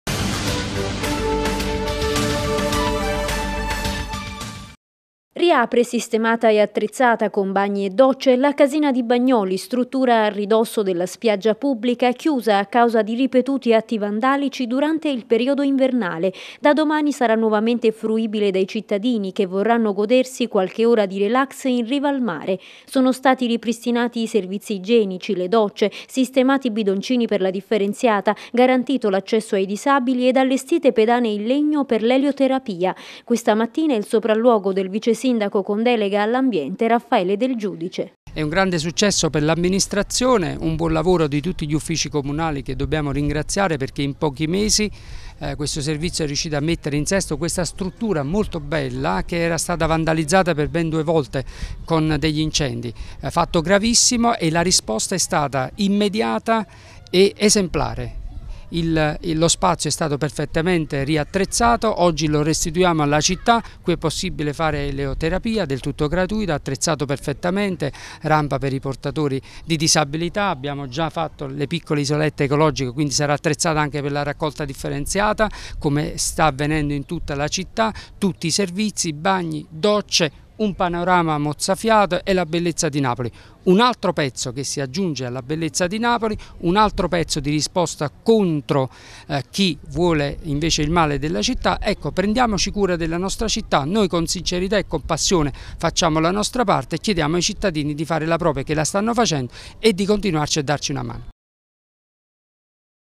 Riapre la casina sulla spiaggia di Bagnoli
Servizi igienici, docce e pedane per l'elioterapia. Da domani riapre la casina sulla spiaggia pubblica di Bagnoli rimessa a posto dopo gli atti vandalici di questo inverno.